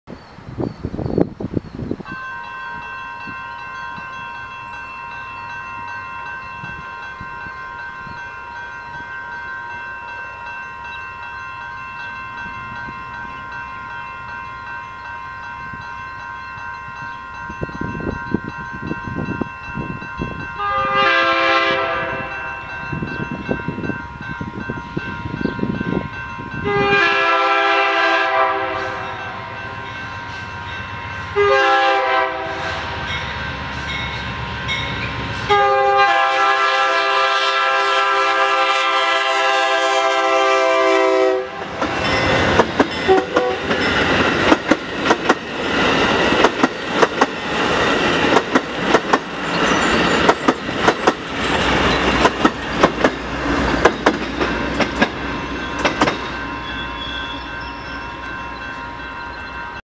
AMTK 126 & 34 lead a WB Amtrak Wolverine on the Michigan Line
AMTK 126 (P42DC) & AMTK 34 (P42DC). 5 Cab Cars. Shot in Wayne, Mi on the Amtrak Michigan Line.